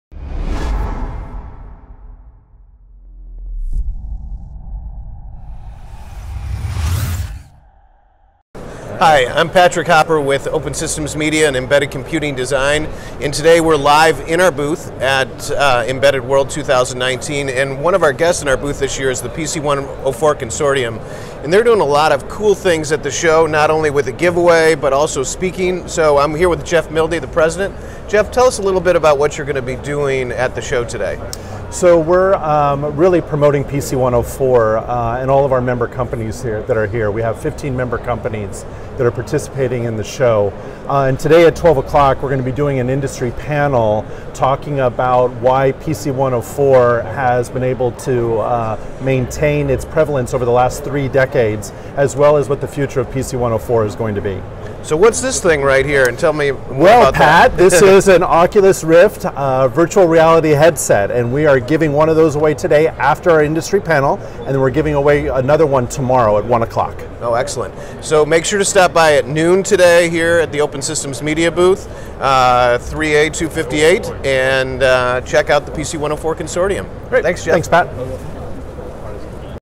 PC/104 Shows Its Strength at Embedded World 2019
Embedded Computing Design's Patrick Hopper talks with the PC/104 Consortium's Jeff Milde at the Embedded Computing Design booth 3A-528.